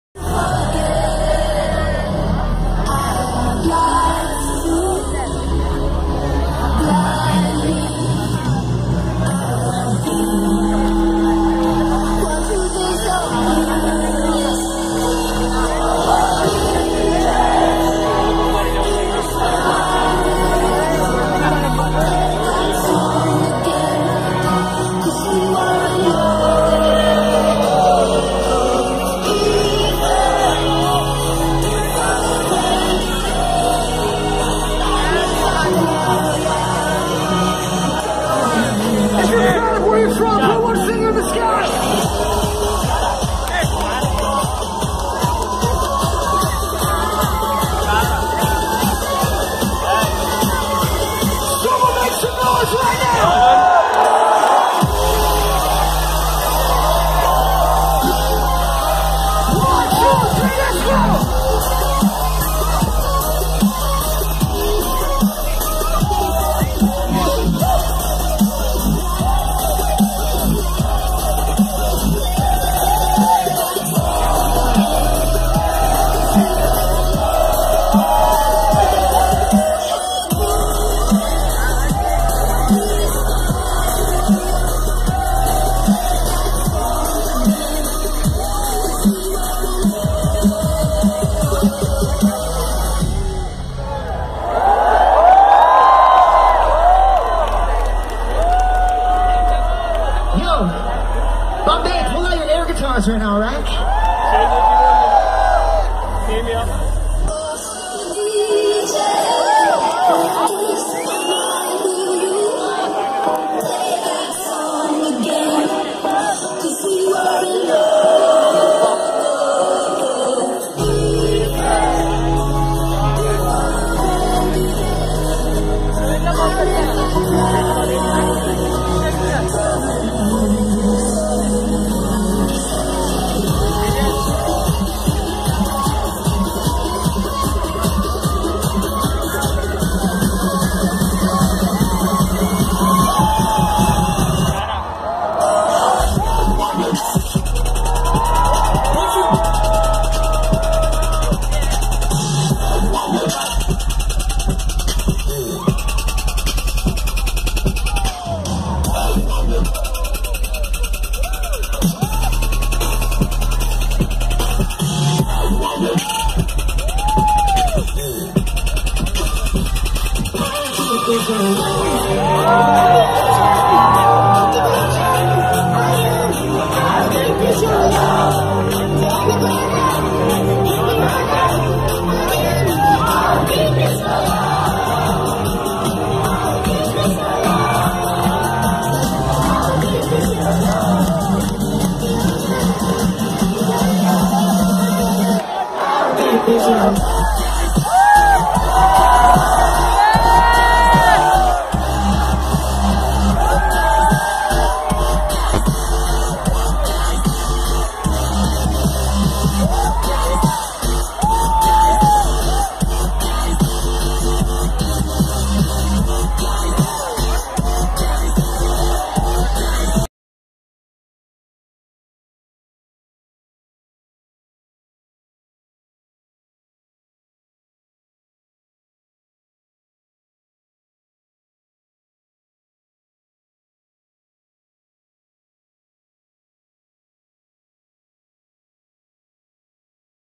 skrIIIex II
First Time in India Vh1 Supersonic brings the maestro of Dubstep
LIVE IN CONCERT 2015
INDIA MUMBAI, RELIANCE JIO GARDENS